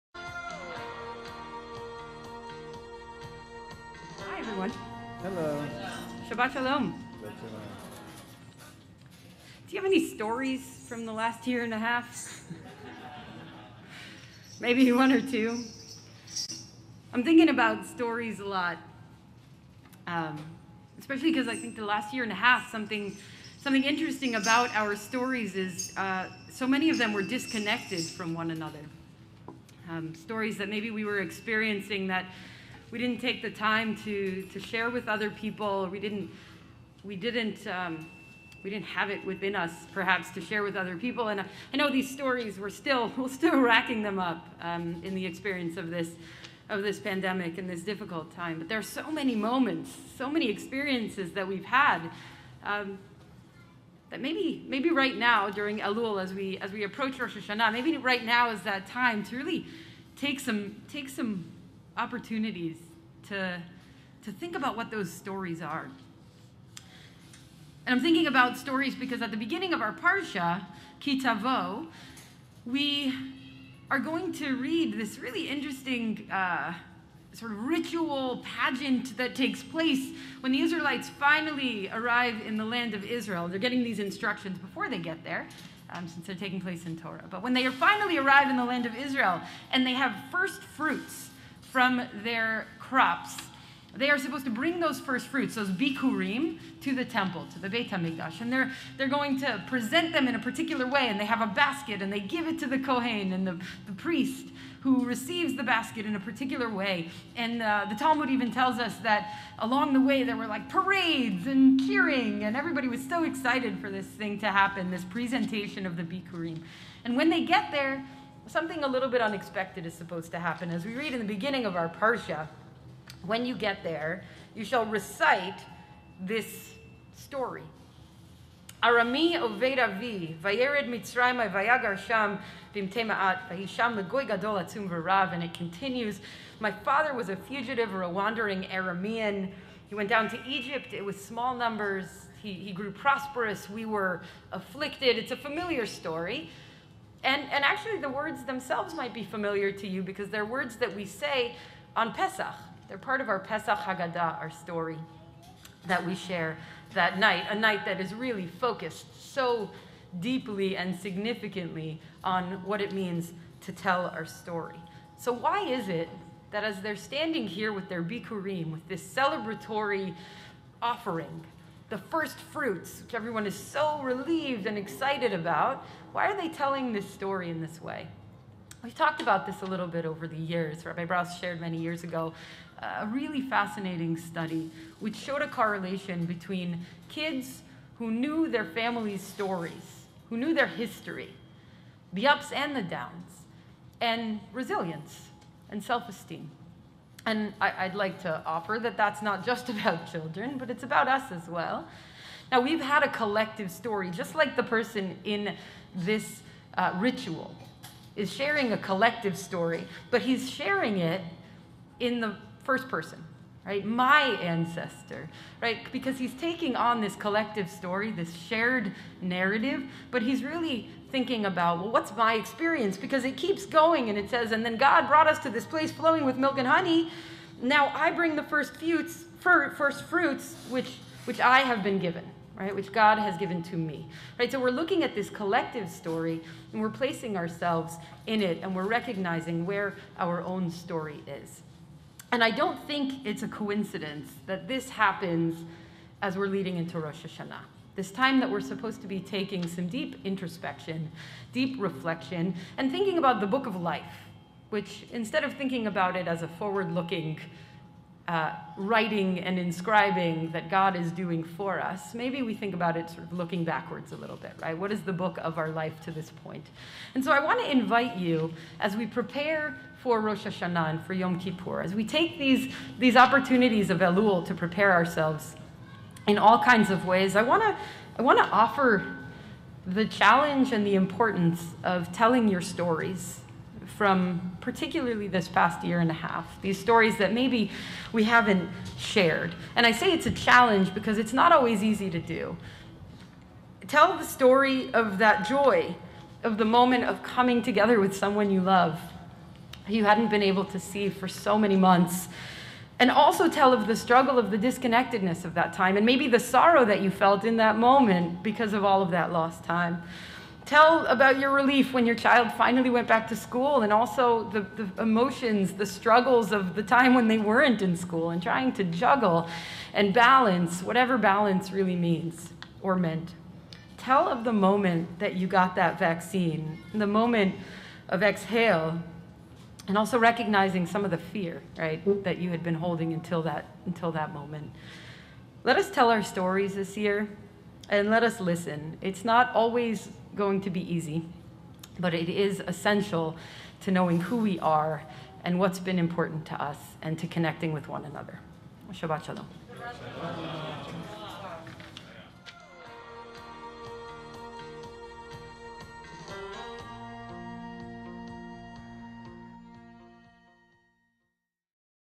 Got Any Stories to Tell? - Rabbi Ronit Tsadok
Ki Tavo 5781 - August 28, 2021:
To show up fully with his bikkurim (first fruits), the Israelite farmer told the story of how he got there. We, too, have stories of highs and lows from this past year and a half, and we may not realize how important it is to share them, especially as prepare for the High Holy Days.